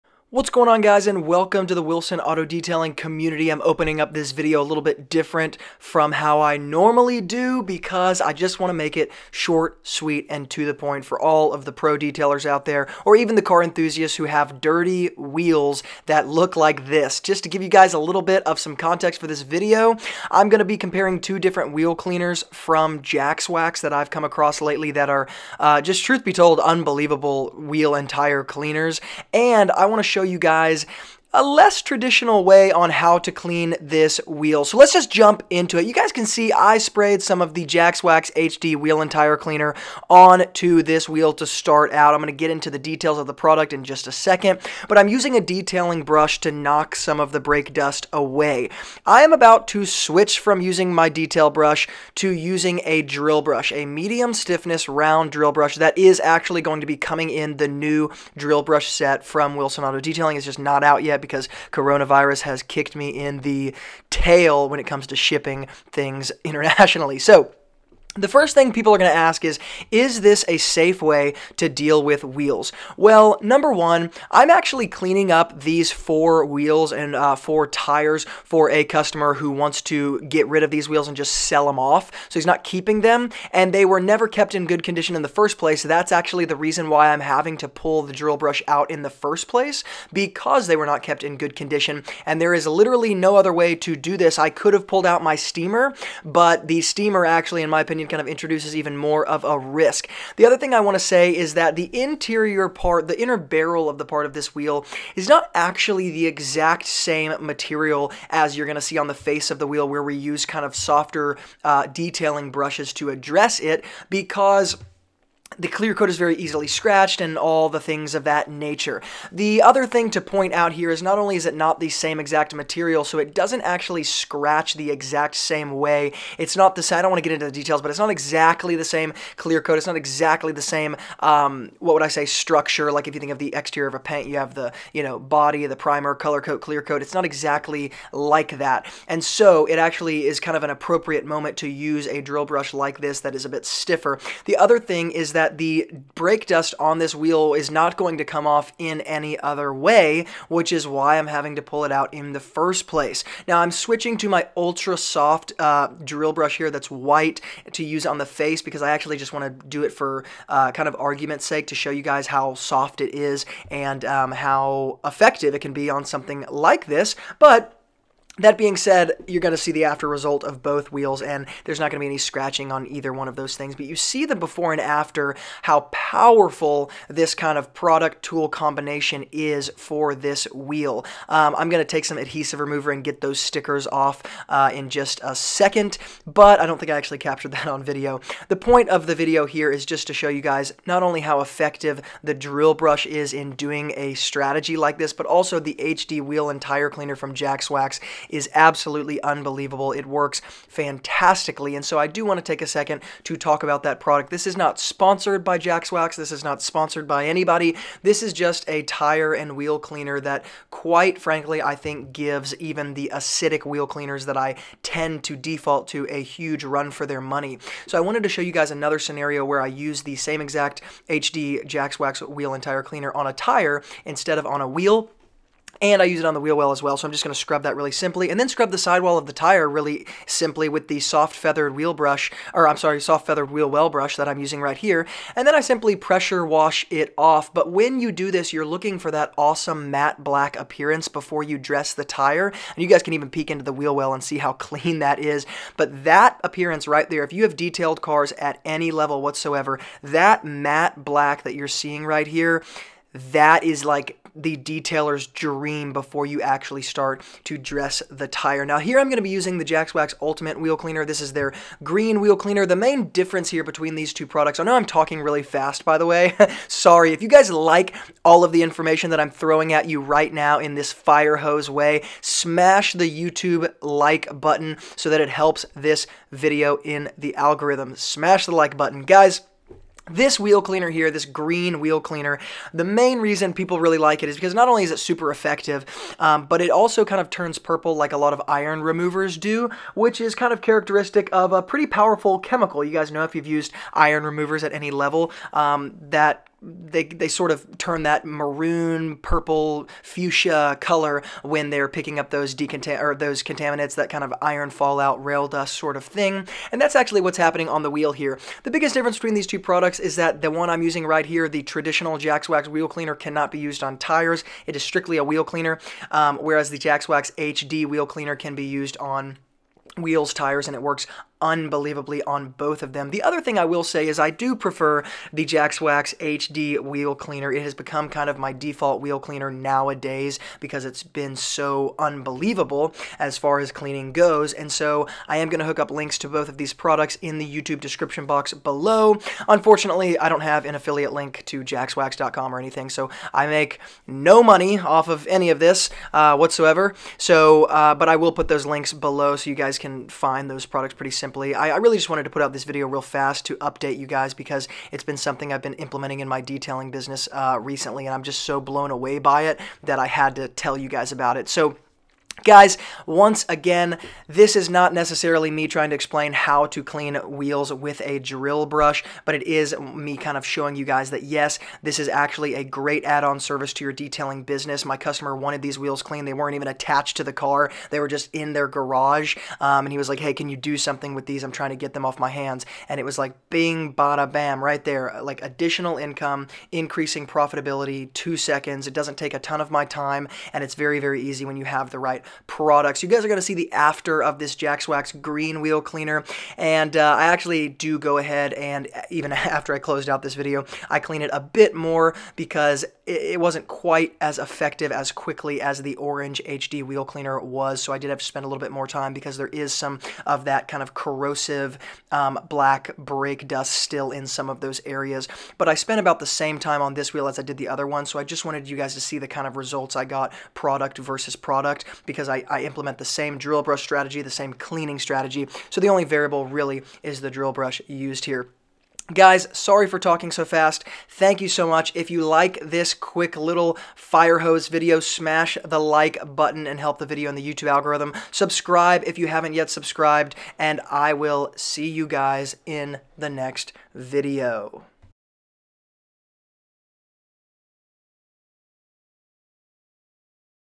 HEAVY Brake Dust Removal & A Hugely Profitable Add On For Detailing Businesses!
HEAVY Brake Dust Removal & A Hugely Profitable Add On For Detailing Businesses! Brake dust removal can be extremely frustrating if you are using the wrong wheel cleaning product and strategy. Not to mention brake dust cleaning can be near impossible sometimes without actually taking the wheel off of the car. Well, here my customer had the wheels taken off for me, AND wanted me to do some extreme brake dust removal as an add on service for him. I'm also using 2 UNBELIEVABLE new wheel cleaning products that you need to check out. Here we go.

General Purpose Drill Brushes
Jax Wax Green Wheel Cleaner
Drill Brushes Medium Stiff
Drill Brushes Soft
Electric Pressure Washer